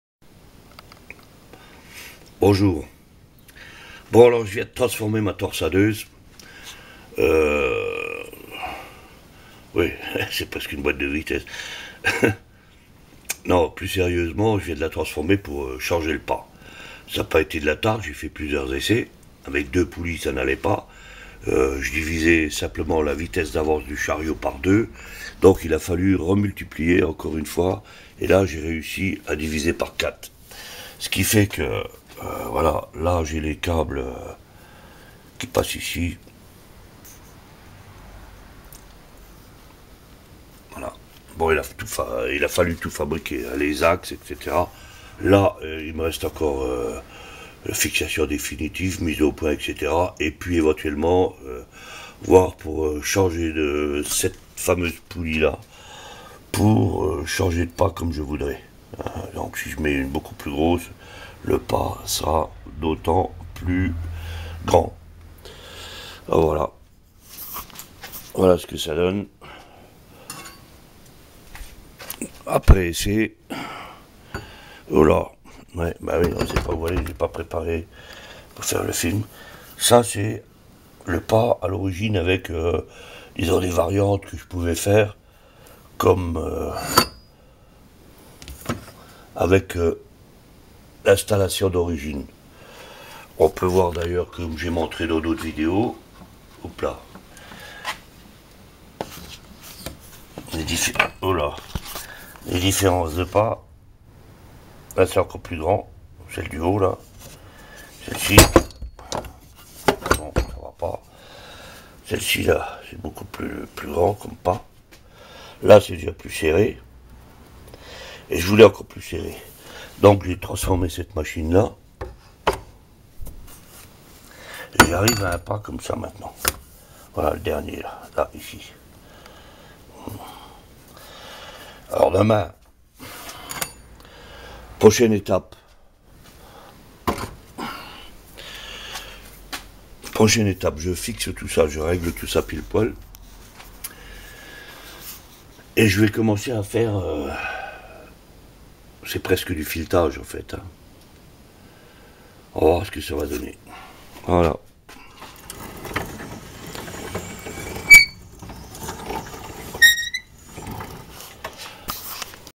transformation de ma torsadeuse pour changer de pas
tournage sur bois machine a faire des torsades